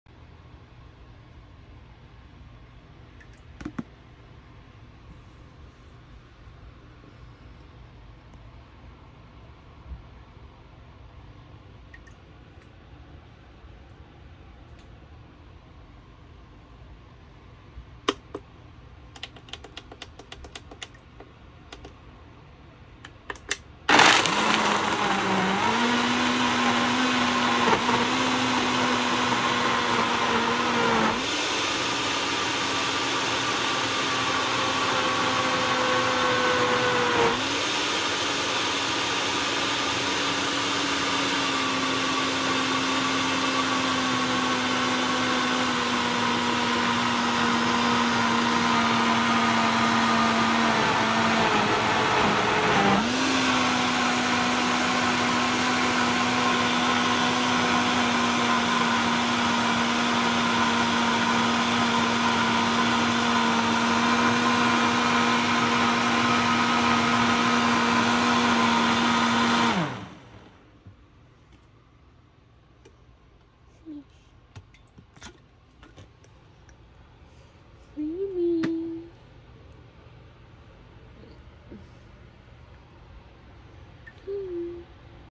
1100 watt,1.5🐴 power 5yr old blender. Liquifies nuts 🌰 n Everything. NO minced fruit 🍉 lol 😂
I can NEVER go back to Weak Blender . Lol evrything blend into smooth  power ! NoT minced fruit 🍉 or nut crumbles !

Daily blends :
Strawberry 🍓, banana 🍌, avocado 🥑, orange 🍊 🌰 nuts 🥜. Yogurt , whey Isolate protein , milk 🥛 , ice 🧊

Heavy dense creamy shake  . U get full usually not hungry for 4 hours lol 😂.  If going walking , I 1.5– 2x scoop protein .  Healthy alternative for Protein granola bar .